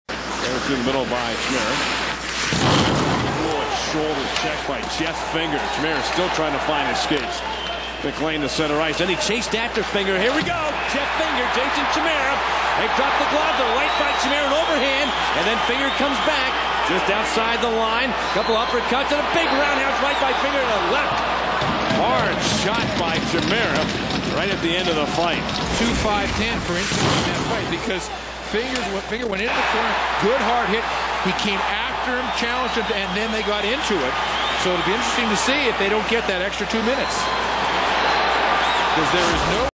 Maple Leafs Sign Jeff Finger
The Toronto Maple Leafs announced Tuesday that the club has signed a 4 year deal with Jeffrey Finger worth 3.5 million a season.